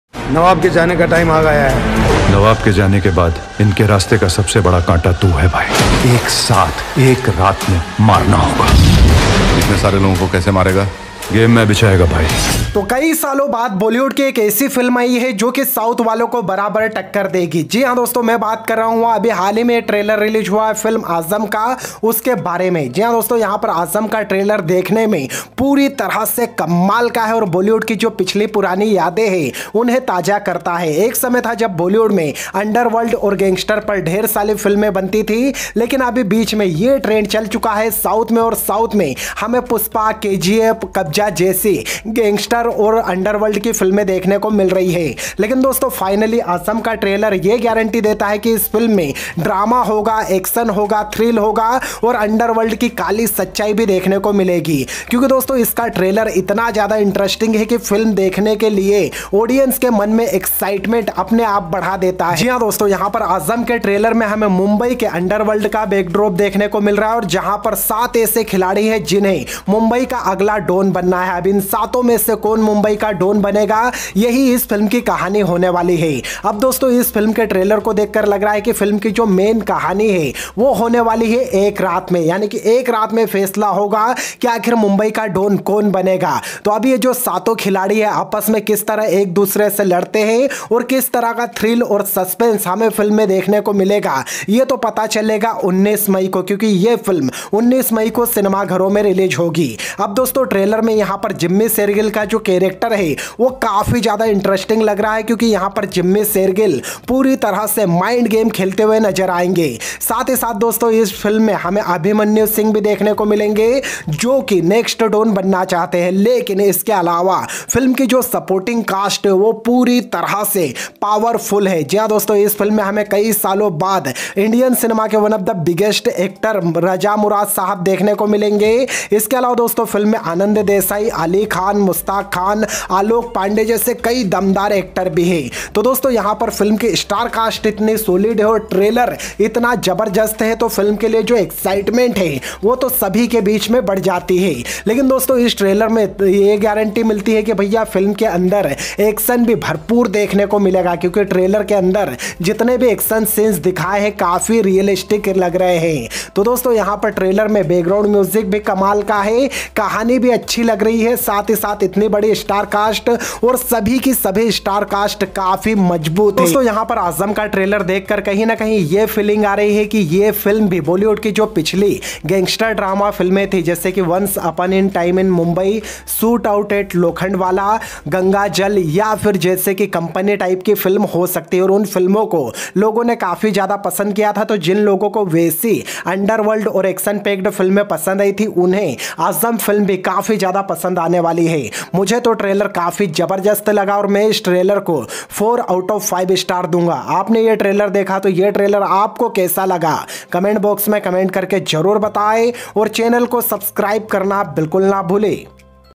Aazam Trailer Review, Jimmy Shergill, Abhimanyu Singh, Raza Murad, KGF2 & Pushpa2 Se Better ??
JIMMY SHEIRGILL - ABHIMANYU SINGH - INDRANEIL SENGUPTA: ‘AAZAM’ TRAILER OUT NOW… 19 MAY RELEASE… JimmySheirgill, AbhimanyuSingh and IndraneilSengupta star in Aazam… Directed by Shravan Tiwari…
 Aazam Movie Trailer Looks More Dangerous Than Allu Arjun Pushpa 2 & Rocking Star Yash KGF 2,
BMX Motion Pictures P Ltd in association with DXB Motion Films LLP presentation… Produced by TB Patel… In *cinemas* 19 May 2023… PVR Pictures nationwide release.